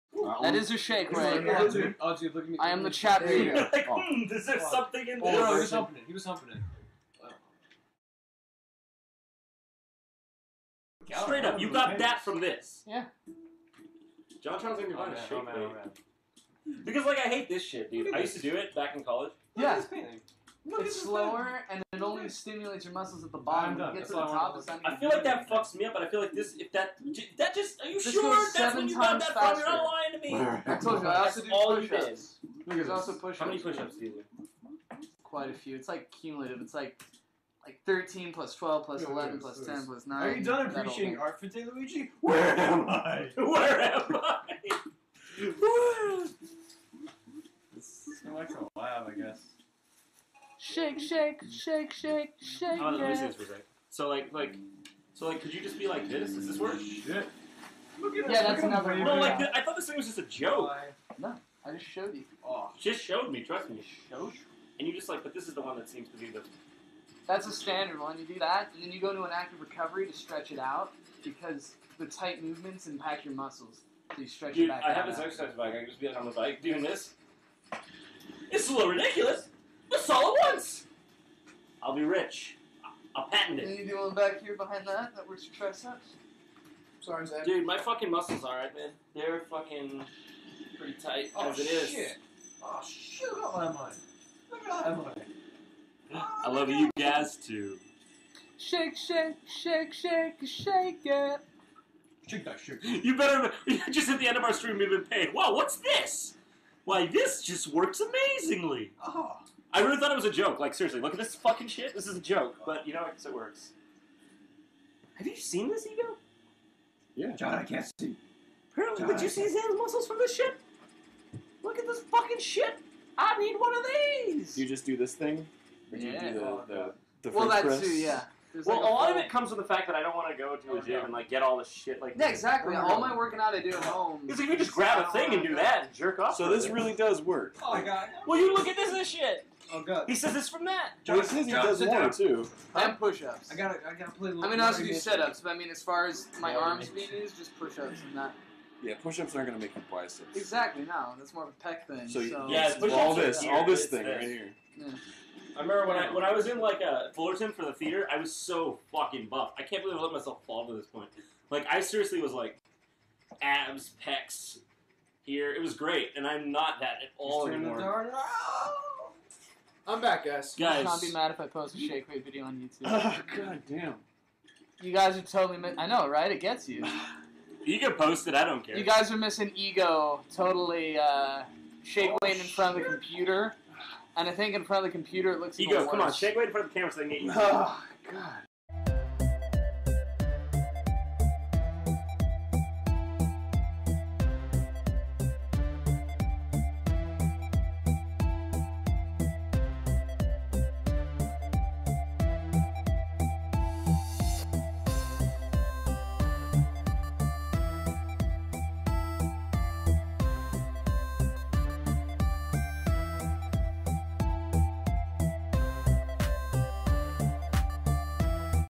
Jontron and Ego play with a Shake-weight (and Barry Speaks)
A video I recorded of Jontron on a video streamed a looooooonnnnngg time ago (before game grumps[TRY August 15, 2011!!]). Also, we hear Barry in this vid. Check out the closed-captions! (took me forever to translate), especially to see what Barry says.

Also, R.I.P. Jon Grump :'( You shall be missed.

Also, The guy on the left (in front of Barry) is a guy youtube content provider who goes by the name of Zanman72. Show'em some love

Also, on another note, it appears that this was filmed in fellow Youtube content provider Jirard Khalil's house, (not sure, just a guess) so I guess I'll provide a link to his channel as well